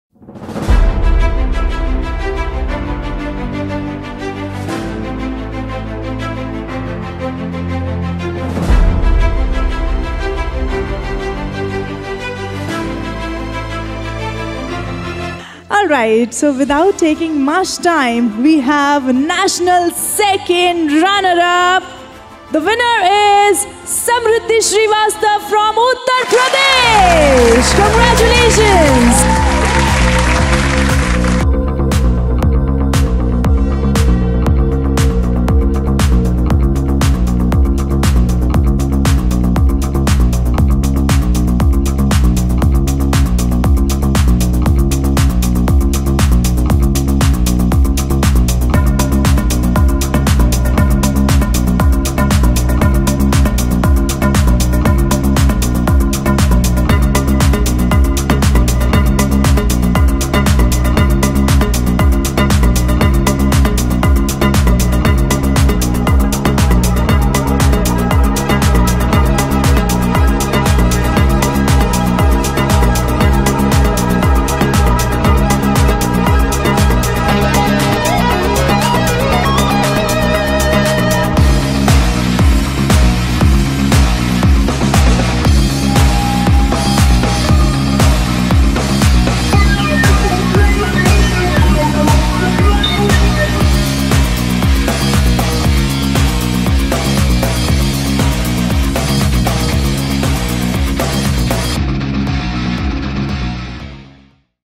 Miss Teen India 2023 Winner Samridhi Srivastava 2nd Runner Up
Samridhi Srivastava has created history with winning Miss Teen India 2023 2nd Runner Up Title at Forever Miss Teen India 2023 National Winner Crowning Ceremony.

Samridhi Srivastava has done well in her Talent Rounds to achieve this feat of his life. Miss Teen India 2023 National Winners Crowning Ceremony held on 23th September 2023 at Marriott Hotel, Jaipur.

Here we bring National Winner Crowning Ceremony video of Samridhi Srivastava (entitled as Miss Teen India 2023 2nd Runner Up), we have Crowned 10+ National Winners from PAN India in the National Winners Crowning Ceremony of Forever Miss Teen India 2023.